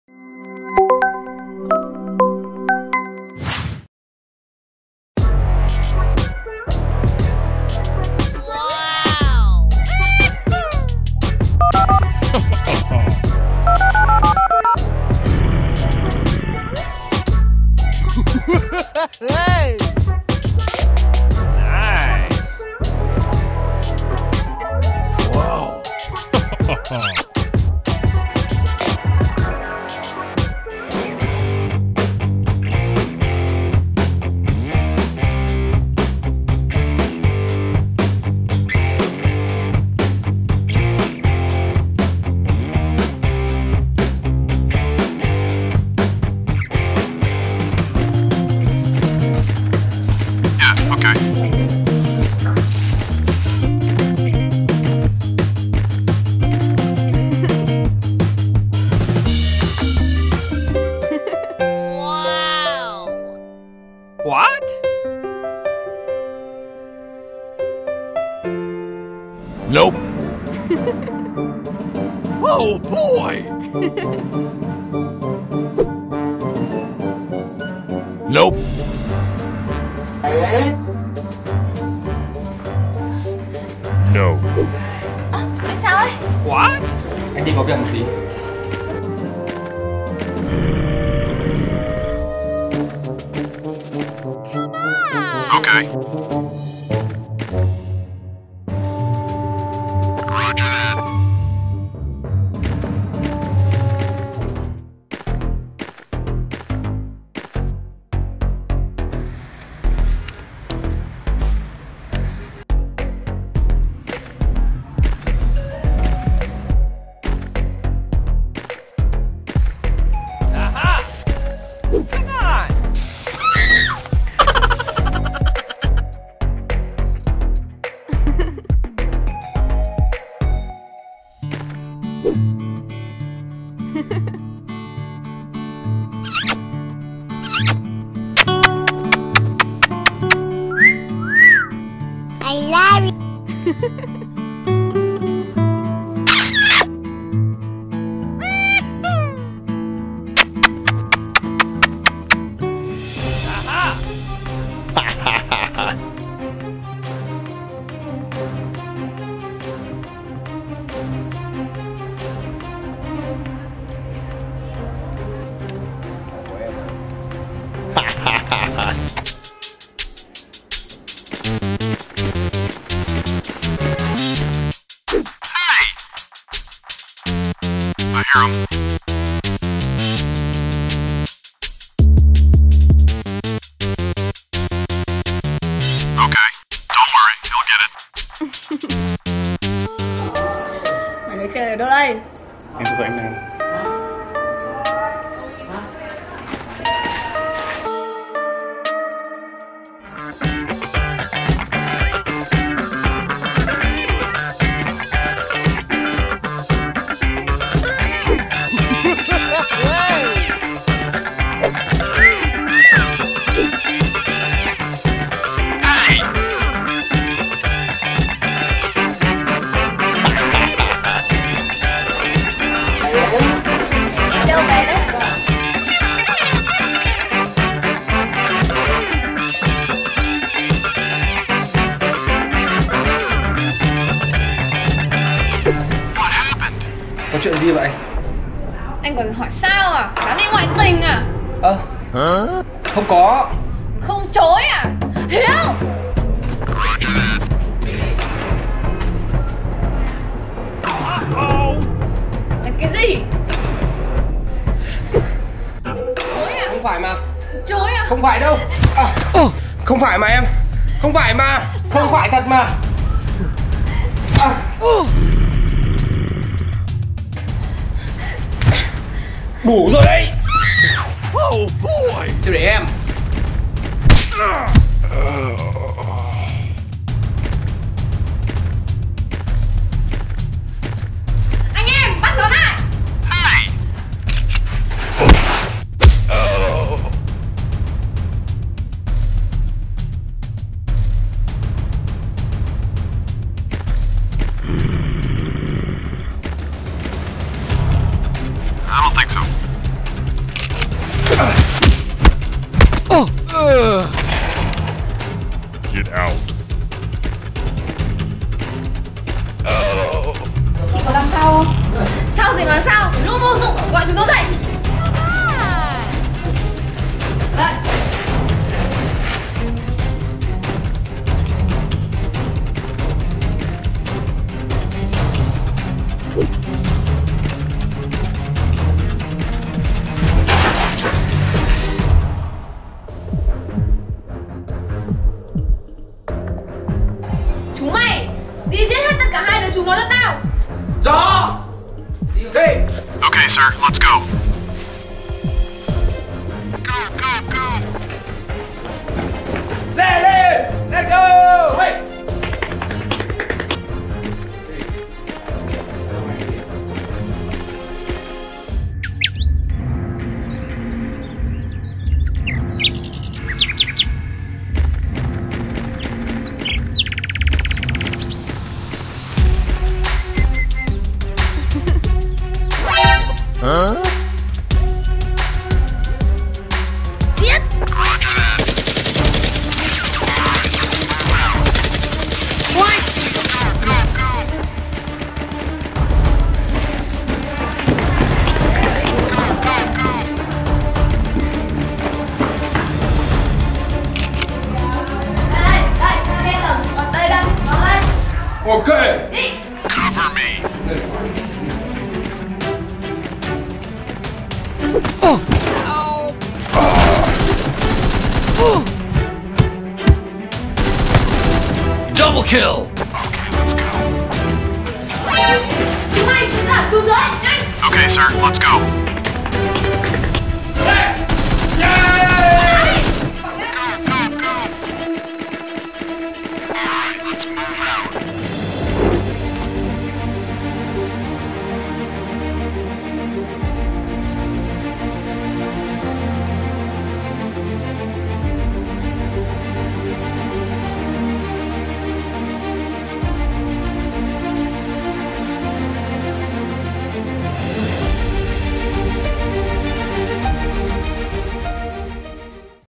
Hihahe Nerf War: Pretty Girl & NYPD Nerf Guns Criminal Group The End Of Love Triangle
Introduce:
Hihahe Nerf Guns Bring you the guns clips Nerf war! In this Nerf War, Team Hero must use new Nerf guns,
Beautiful costumes, special team costumes and cosplay, fighting in a Nerf battle for Task Force & criminal groups.

Hastag